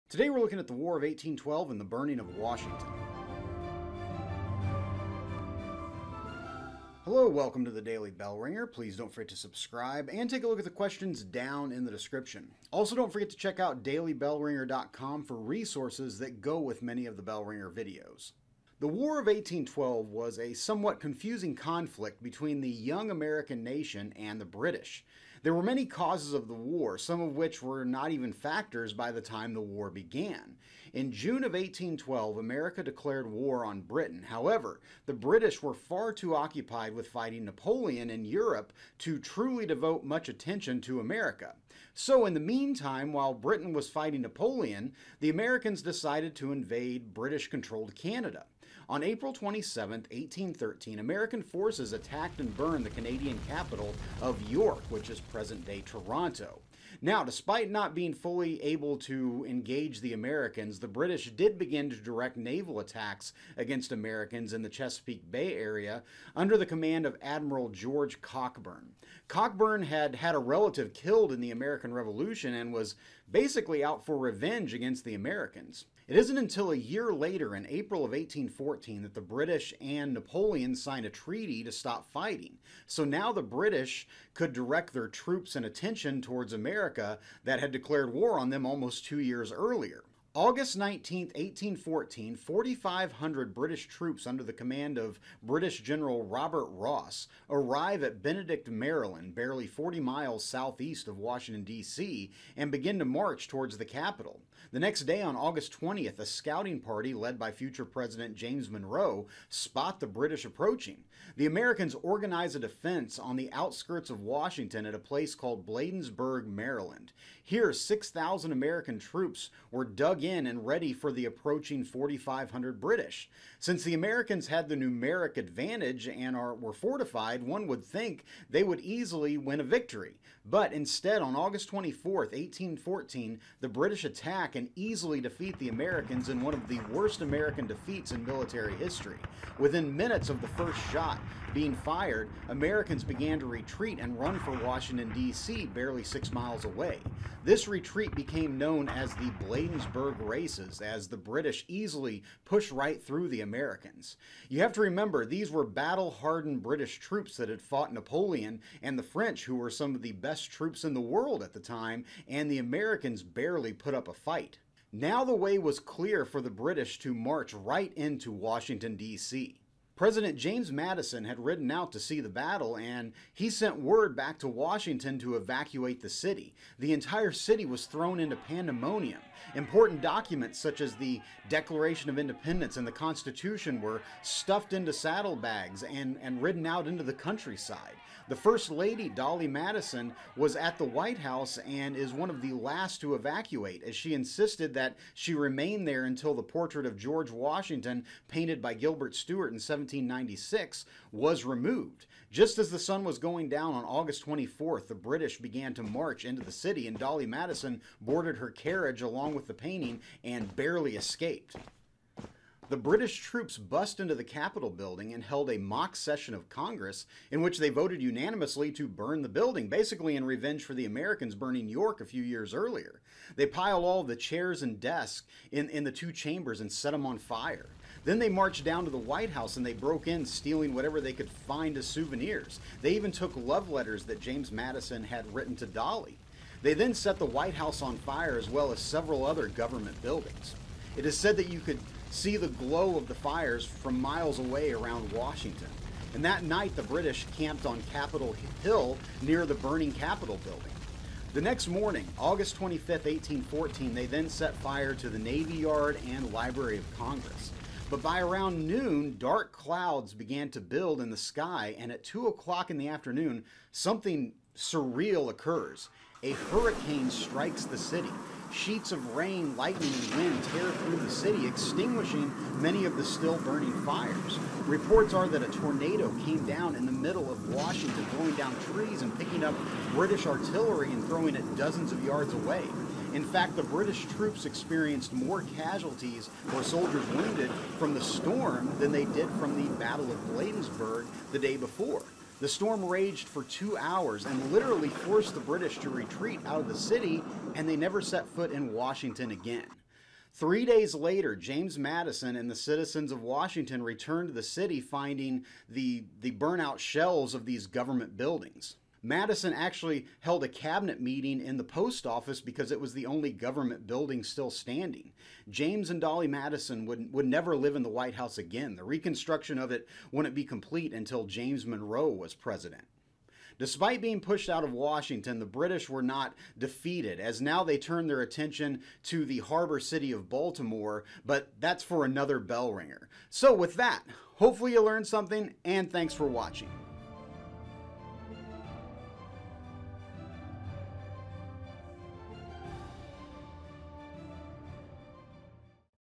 War of 1812 Burning of Washington | Daily Bellringer
War of 1812 Burning of Washington: The British march into the nation's capital and set fire to the city. It looks like all is lost but an unlikely event causes the British to retreat out of the city. Questions below:

1. What is the bay near Washington D.C. that Admiral Cockburn and the British terrorize in 1813?
2. Who led the scouting party that spotted the British marching towards Washington?
3. What did Dolley Madison stay in the White House for to try to save?
4. What forces the British to retreat out of Washington?
5. Some suggested to Madison that the capital be relocated but he refused. If you were in charge where would you move the capital of the country to and why?

**ADDITIONAL MATERIALS AND WORKSHEETS FOR MANY OF THE BELLRINGER VIDEOS